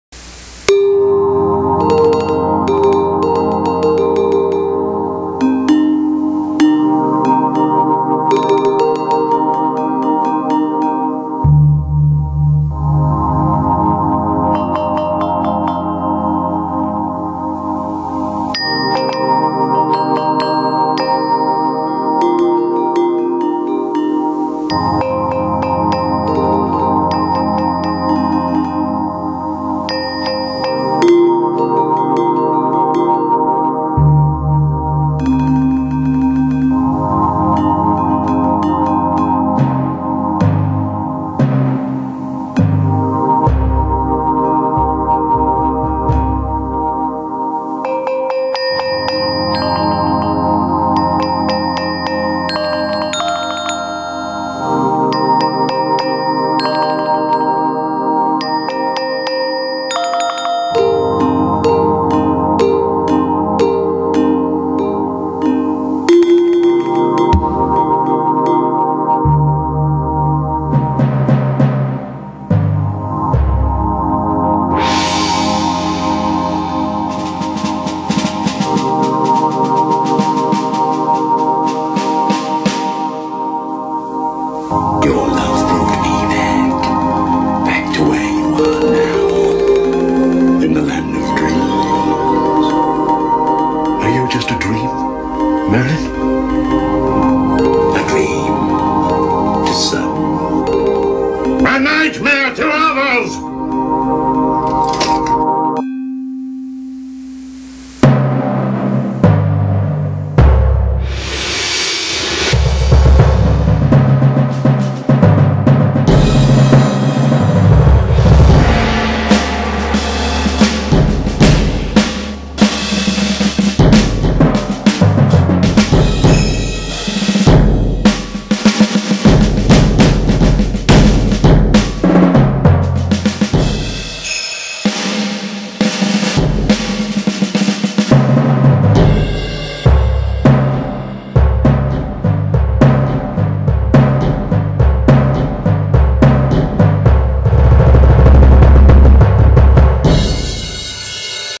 Some Balinese and Pagan love music
mixing symohonic and Gamelan bells , with a special guest on vocals .